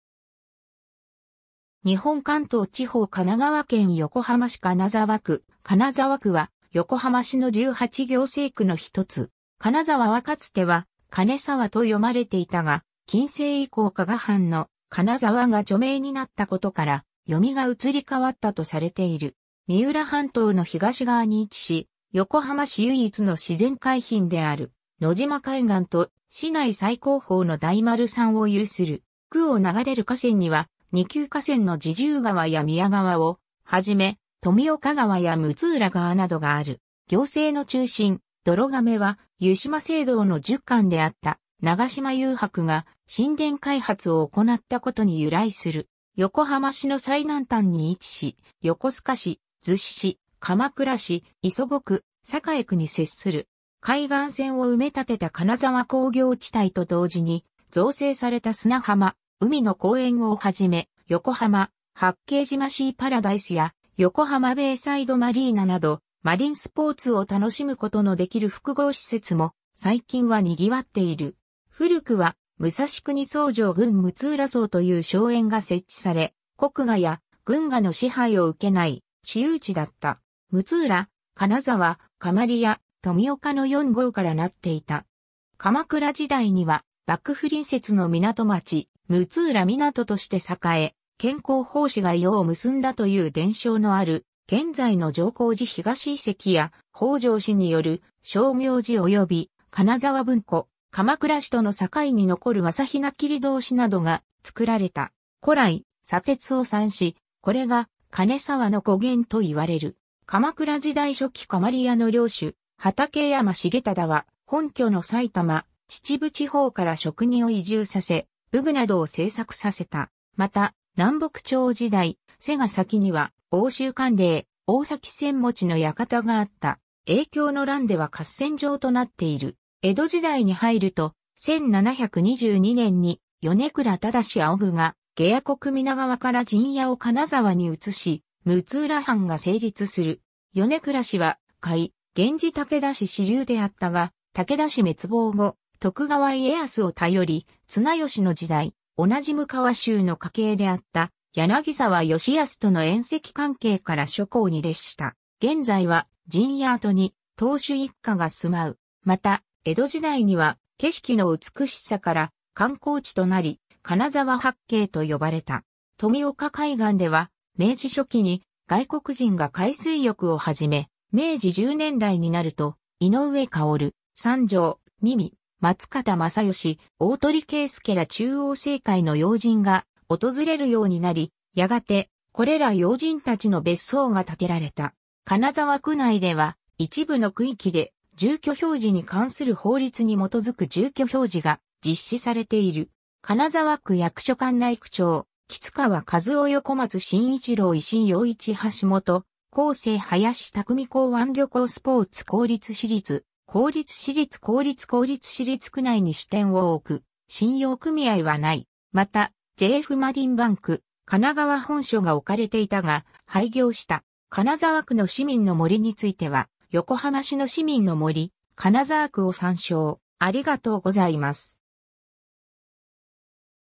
金沢区
日本 」 関東地方 」 神奈川県 」 横浜市 」 金沢区 金沢区（かなざわく）は、横浜市の18行政区の一つ。
 「金沢」はかつては「かねさわ」と読まれていたが、近世以降加賀藩の「かなざわ」が著名になったことから、読みが移り変わったとされている。
 三浦半島の東側に位置し、横浜市唯一の自然海浜である野島海岸と、市内最高峰の大丸山を有する。
区を流れる河川には、二級河川の侍従川や宮川を始め、富岡川や六浦川などがある。
 行政の中心・泥亀は、湯島聖堂の儒官であった永島祐伯（号を泥亀）が新田開発を行ったことに由来する。
横浜市の最南端に位置し、横須賀市、逗子市、鎌倉市、磯子区、栄区に接する。
海岸線を埋め立てた金沢工業地帯と同時に造成された砂浜「海の公園」をはじめ、横浜・八景島シーパラダイスや横浜ベイサイドマリーナなど、マリンスポーツを楽しむことのできる複合施設も最近は賑わっている。
 古くは武蔵国倉城郡（後の久良岐郡）六浦荘という荘園が設置され、国衙（こくが）や郡衙の支配を受けない私有地だった。
六浦・金沢（かねさわ）・釜利谷・富岡の4郷から成っていた。
（金沢文庫#「かねさわ」と「かなざわ」も参照）。
鎌倉時代には幕府隣接の港町・六浦湊として栄え、兼好法師が庵を結んだという伝承のある現在の上行寺東遺跡や、北条氏による称名寺及び金沢文庫、鎌倉市との境に残る朝比奈切通し（国の史跡）などが造られた。
古来、砂鉄を産し、これが「かねさわ」の語源といわれる。
 鎌倉時代初期釜利谷の領主・畠山重忠は、本拠の埼玉・秩父地方から職人を移住させ、武具などを製作させた。
また、南北朝時代、瀬ヶ崎には奥州管領・大崎詮持の居館があった。
永享の乱では合戦場となっている。
 江戸時代に入ると、1722年に米倉忠仰が下野国皆川から陣屋を金沢に移し、六浦藩が成立する（横浜市域では唯一の大名）。
米倉氏は甲斐源氏武田氏支流であったが武田氏滅亡後、徳川家康を頼り、綱吉の時代、同じ武川衆の家系であった柳沢吉保との縁戚関係から諸侯に列した。
現在は陣屋跡に当主一家が住まう。
また江戸時代には景色の美しさから観光地となり金沢八景と呼ばれた。
 富岡海岸では明治初期に外国人が海水浴を始め、明治10年代になると井上馨、三条実美、松方正義、大鳥圭介ら中央政界の要人が訪れるようになり、やがてこれら要人たちの別荘が建てられた。
 金沢区内では、一部の区域で住居表示に関する法律に基づく住居表示が実施されている。
 金沢区役所管内（101町丁） 区長　 　橘川 和夫 （ - 2005年3月31日） 　横松 進一郎（2005年4月1日 - 2008年3月31日） 　石井 洋一 （2008年4月1日 - 2010年3月31日） 　橋本 康正 （2010年4月1日 - 2012年3月31日） 　林 琢己　　（2012年4月1日 - ） 港湾 漁港 スポーツ 公立 私立 公立 私立 公立 公立 私立 区内に支店を置く信用組合はない（信用組合横浜華銀などは営業区域内である）。
また、JFマリンバンク神奈川本所（神奈川県信用漁業協同組合本所）が置かれていたが、廃業した。
 金沢区の市民の森については「横浜市の市民の森#金沢区」を参照